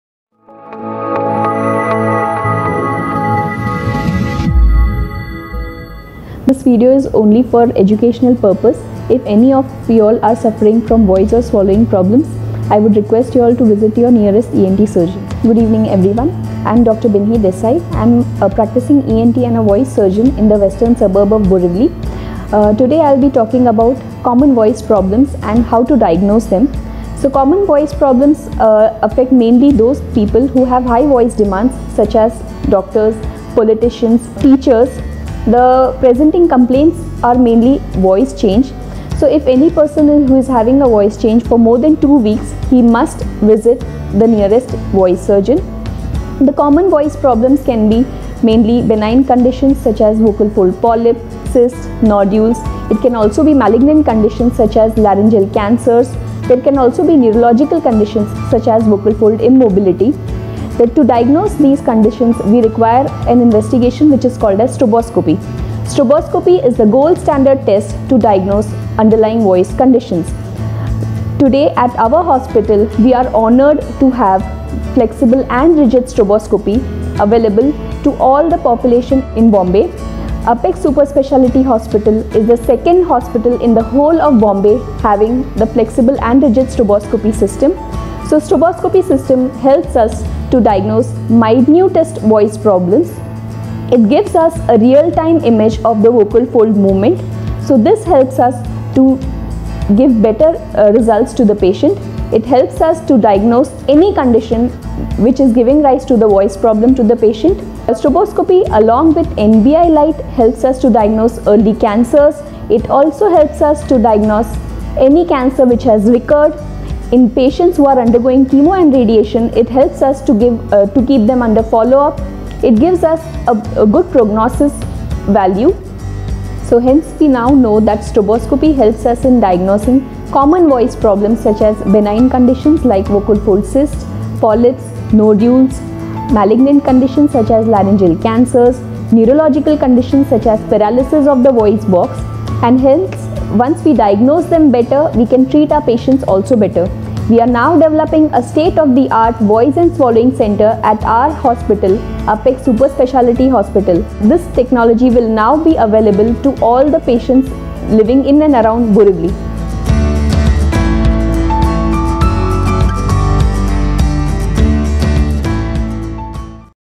Know more about STROBOSCOPY.
STROBOSCOPY SYSTEM IS NOW AVAILABLE AT APEX SUPERSPECIALITY HOSPITAL BORIVALI WEST.

NOW WE HAVE A STATE-OF-THE ART DEDICATED VOICE AND SWALLOWING CENTER IN BORIVALI FOR ALL OUR PATIENTS .
TEAM OF
1. A LARYNGOLOGIST-  DR BINHI. H. DESAI AND
SPEECH AND SWALLOWING THERAPISTS FROM ANDHERI TO VIRAR to serve our patients better.

Now we have the LATEST AND THE BEST OLYMPUS STROBOSCOPY SYSTEM installed for all our patients at Borivali

call on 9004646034/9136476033/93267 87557  for more information.